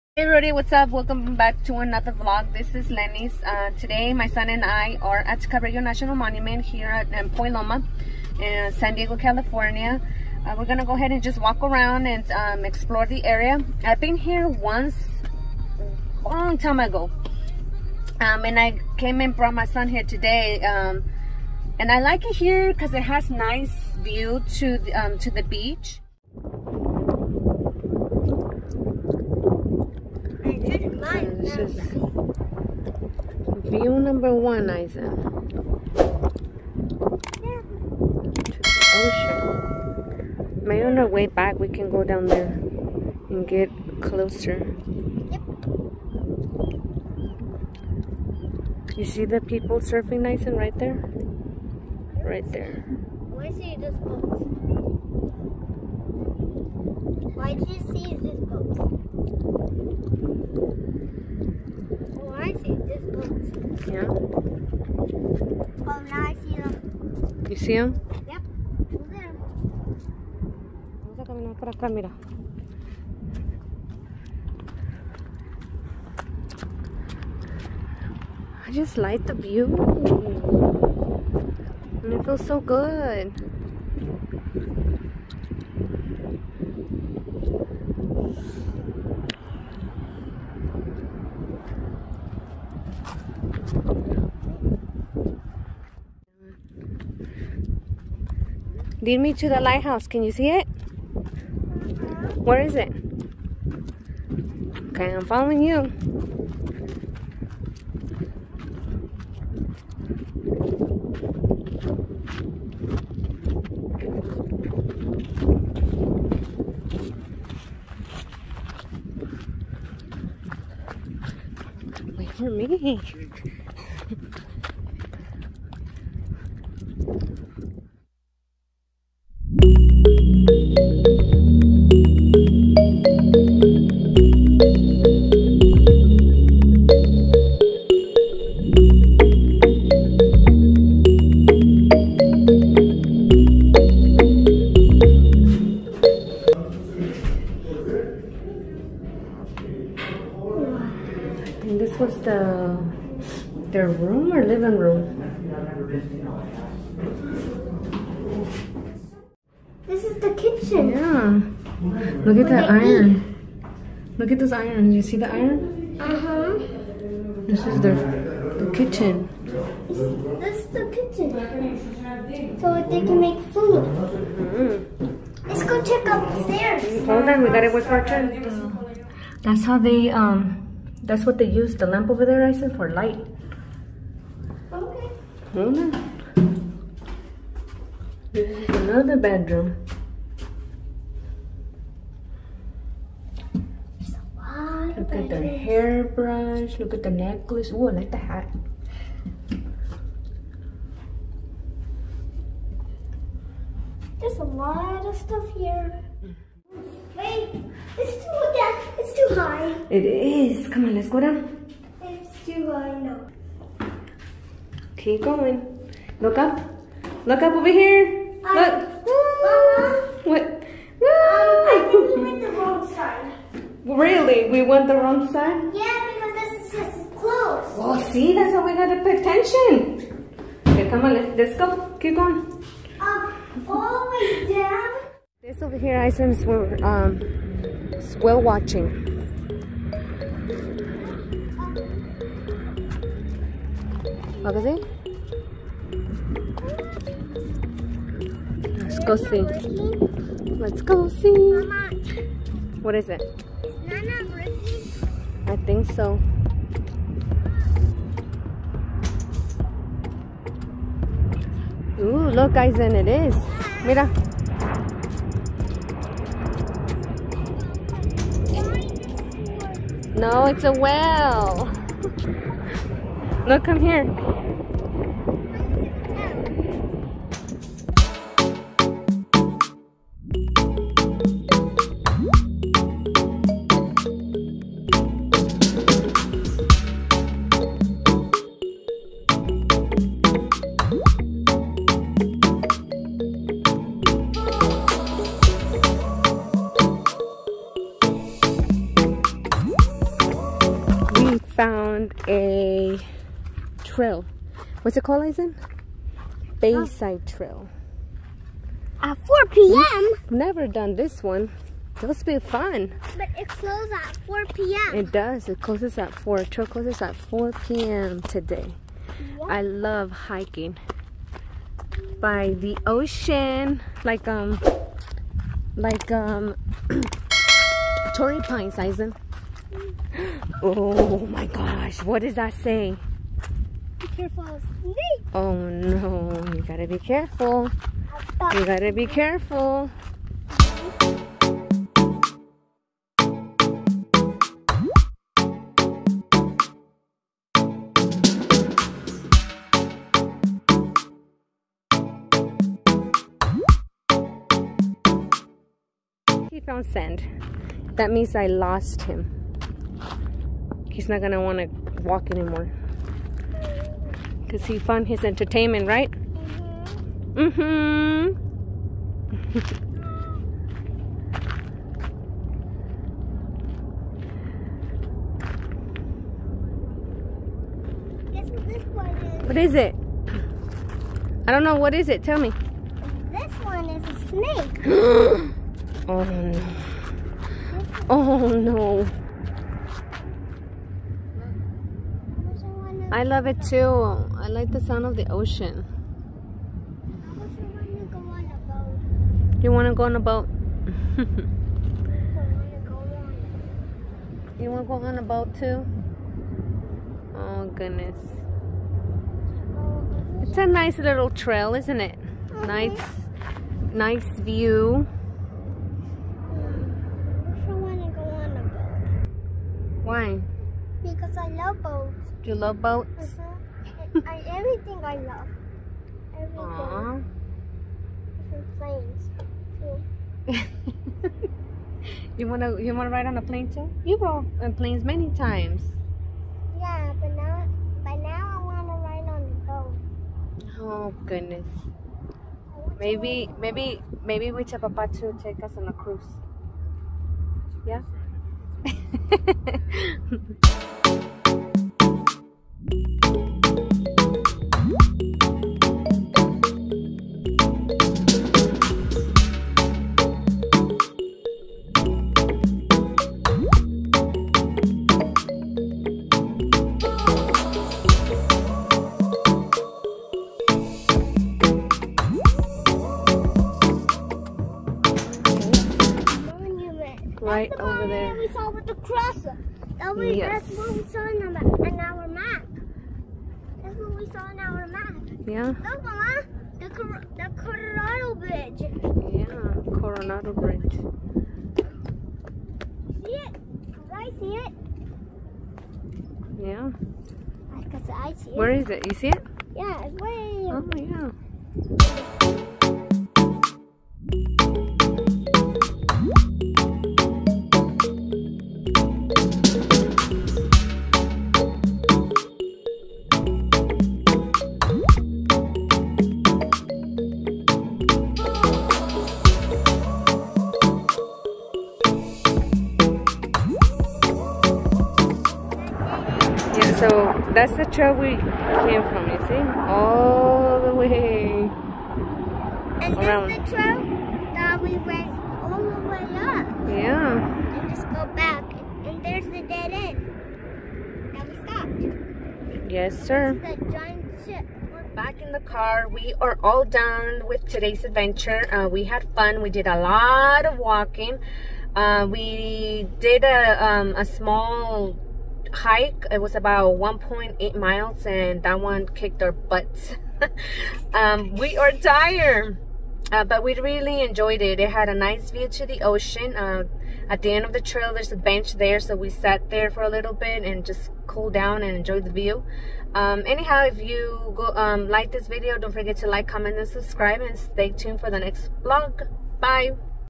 Cabrillo National Monument Point Loma San Diego CA| short hike with beautiful view| Lighthouse tour
Cabrillo National Monument is at the southern tip of the Point Loma Peninsula in San Diego, California. It commemorates the landing of Juan Rodríguez Cabrillo at San Diego Bay. As for the history of this monument: On September 28, 1542, Juan Rodriguez Cabrillo landed in San Diego. This event marked history as it would be the first time that a European expedition had come on what later became the west coast of the US. Cabrillo accomplishments were memorialized on October 14, 1913 with the establishment of Cabrillo National Monument. A statue and museum in the Visitor Center commemorate Juan Rodriguez Cabrillo's exploration of the coast of California.

In a former army building an exhibit tells the story of the coast artillery on Point Loma. In the winter, migrating gray whales can be seen off the coast but their migration pick is in mid January. Bayside Trail offers a quiet place to relax. Park has beautiful view to the ocean and a lot of history to say the least. Take a look inside the lighthouse and walk around visit the military exhibits. Don't forget to visit the visitors center it's a small museum inside, my son and I didn't get to go inside because we were tired of all the walking.